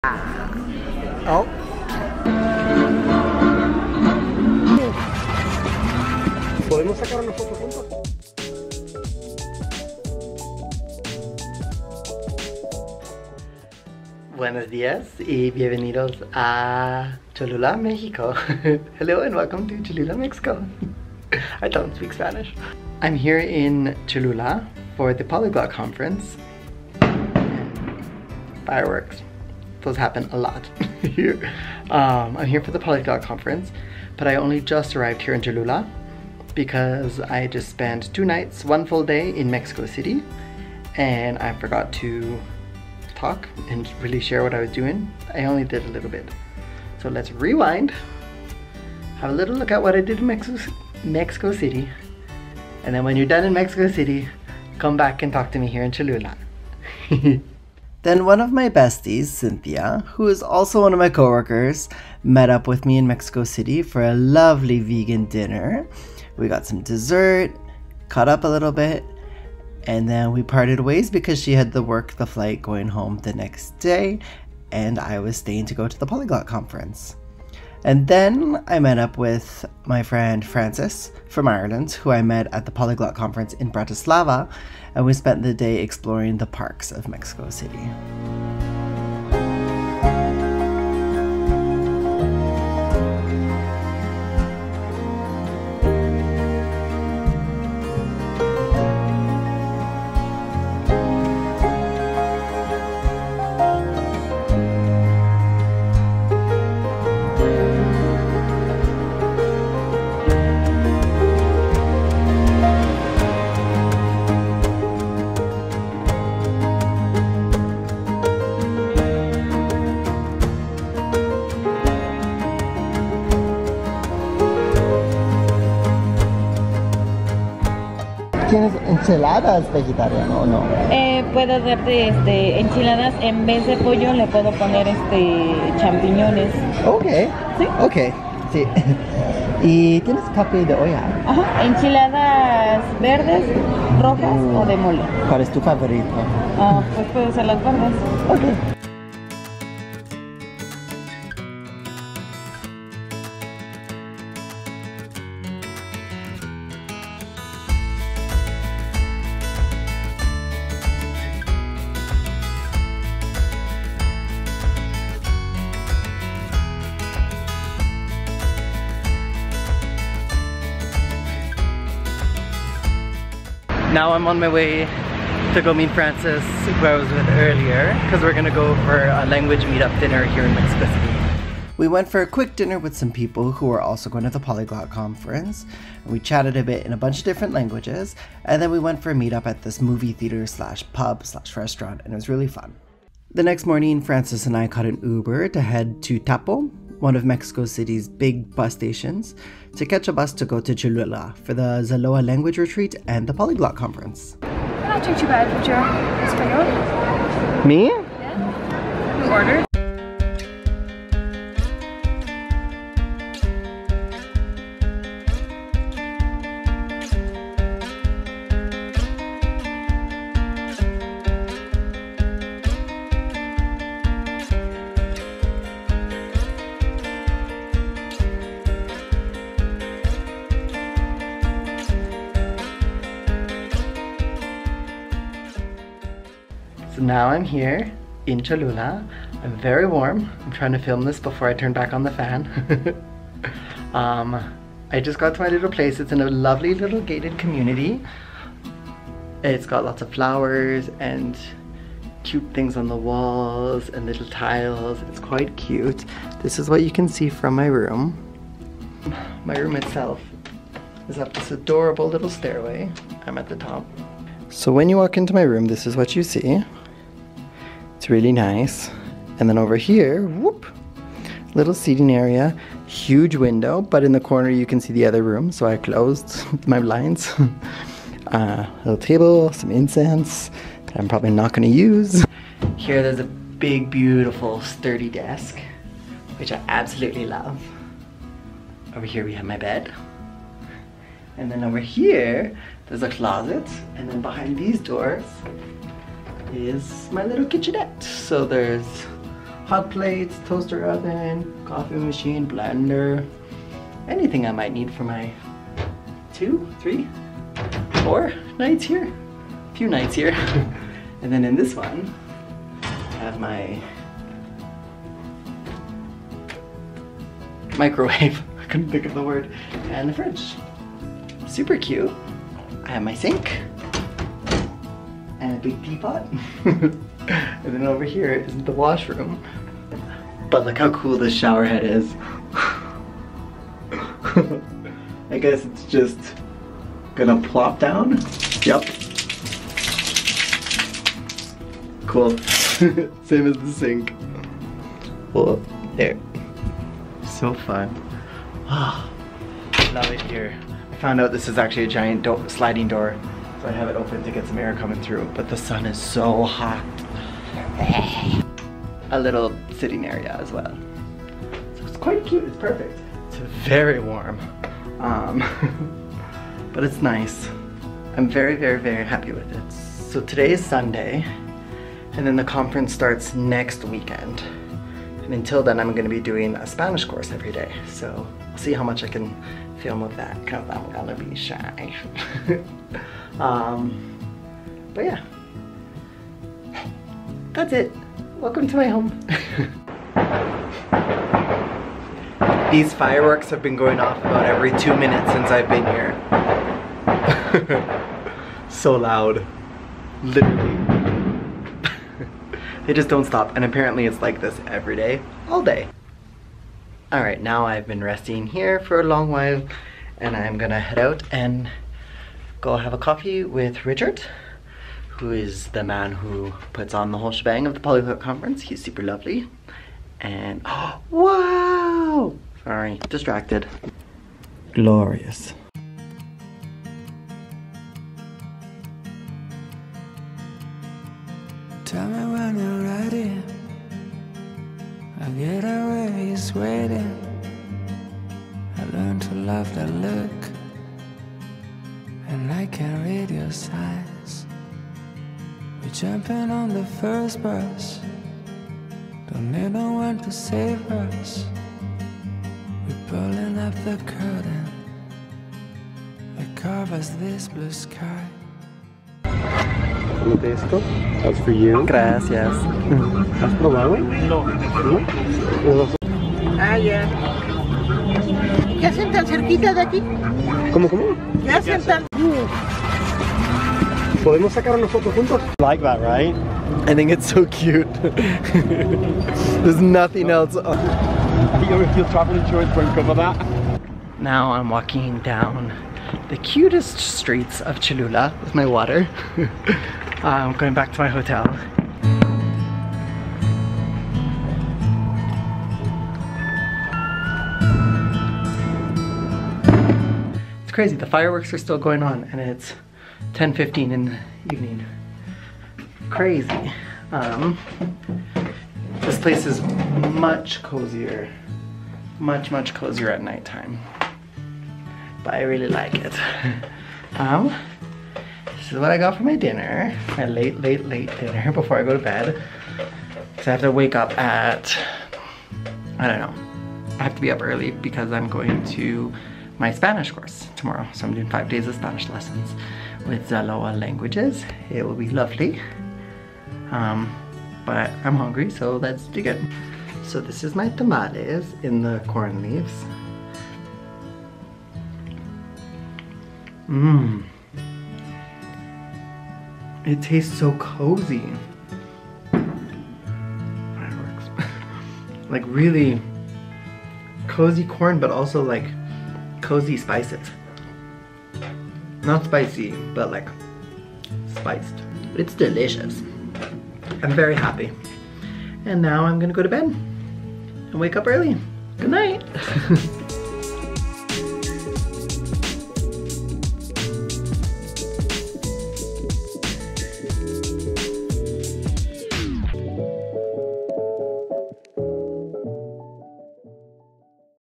Arriving to Mexico City and Cholula for the Polyglot Conference
Follow me in the daily life of a flight attendant polyglot.

I had arrived in Mexico City and made my way to Cholula for the Zaloa Languages Spanish retreat and the Polyglot Conference :)

00:00 Introduction
00:14 Welcome to Cholula!
00:57 Dinner with Cinthia in CDMX
01:17 Exploring Mexico City
03:14 Mexico City to Cholula
03:58 Apartment Tour
07:05 Exploring Cholula